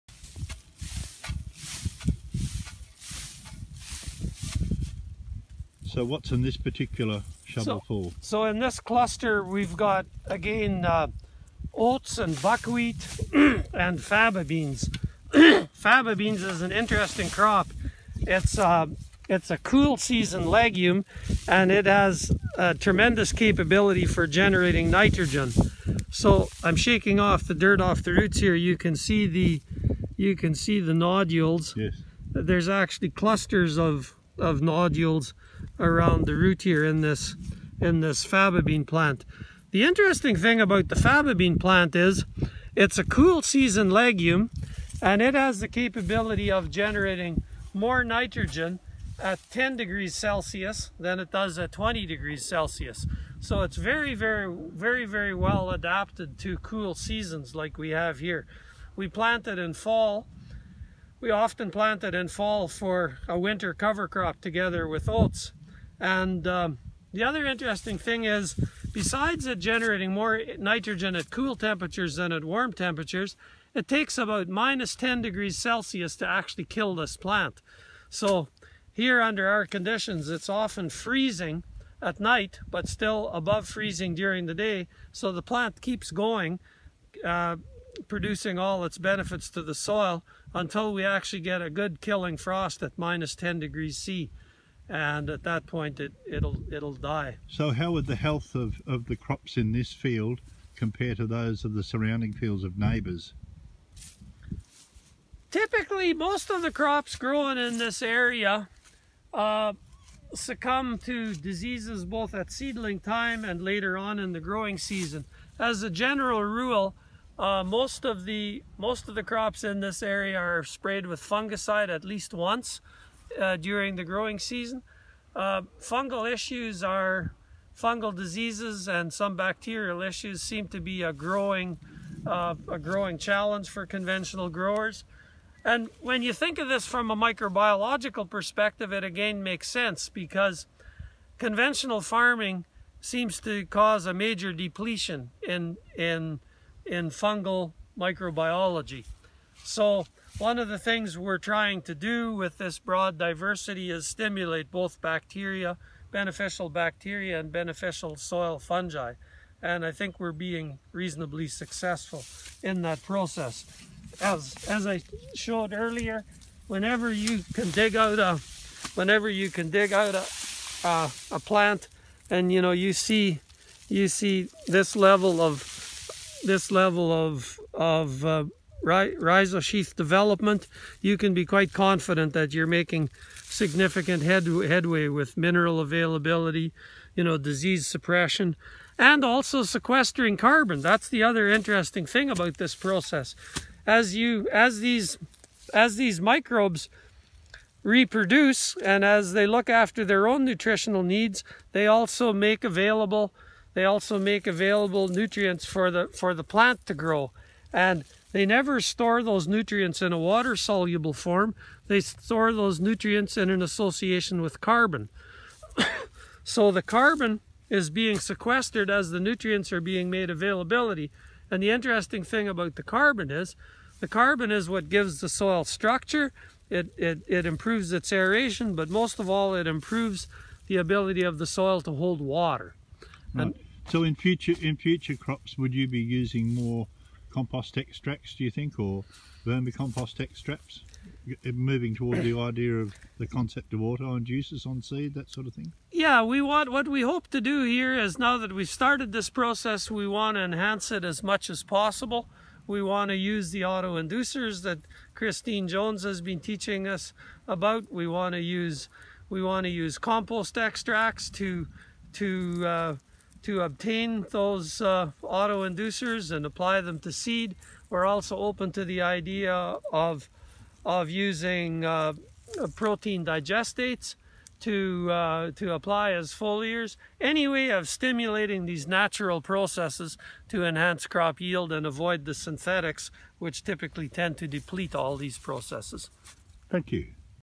Gerald Wiebe Part 2 - Rebuilding soils without chenistry
Chemical free soil reconstruction of a prevously damaged soil